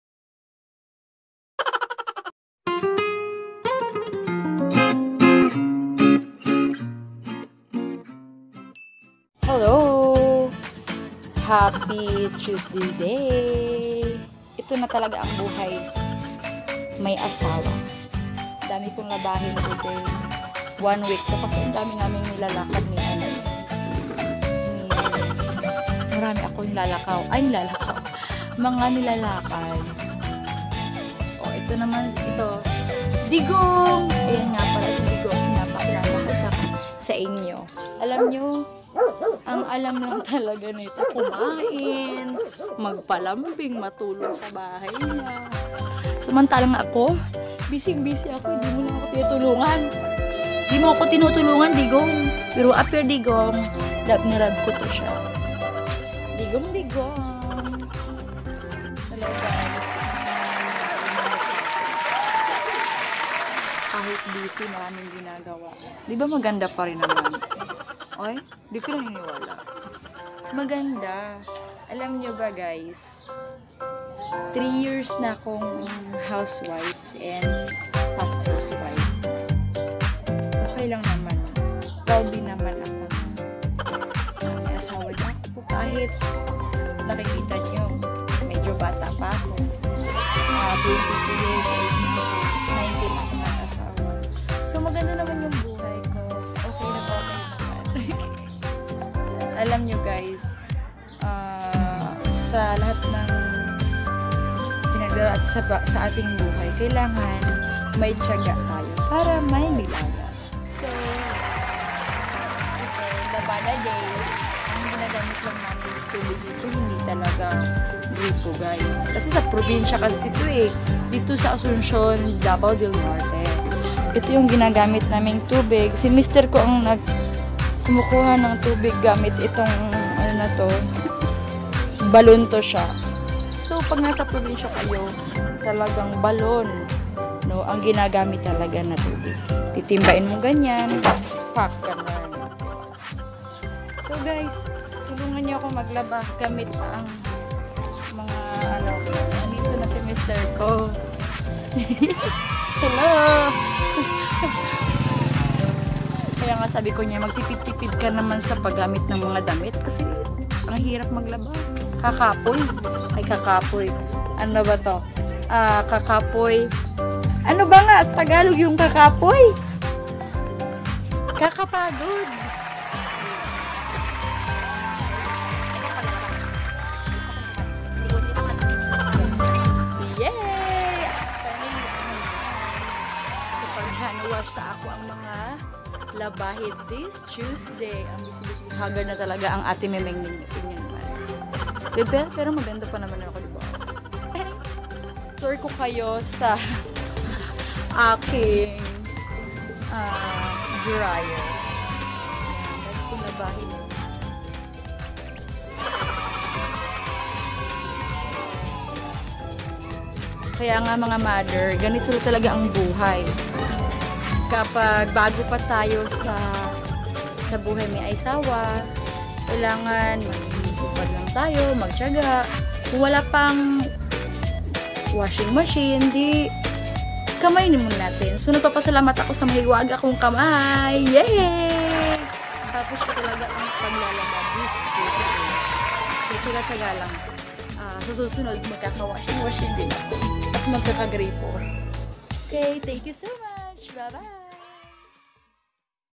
Sa mga Walang Washing machine Tara usap tayo//April//Episode 5
Gusto lang po i share ang tuesday ko sa inyu.
Siguro yung iba nakakarelate na hindi madaling maglaba yung mga nag ha-hand washing lang at gamit ang balon na tubig..
Kaya naman antay-antay lang tayo na magka washing machine
Mga momshieee. At yung mga bago lang sa married life go! Lang tayo. ❤❤

Thank you kapatid.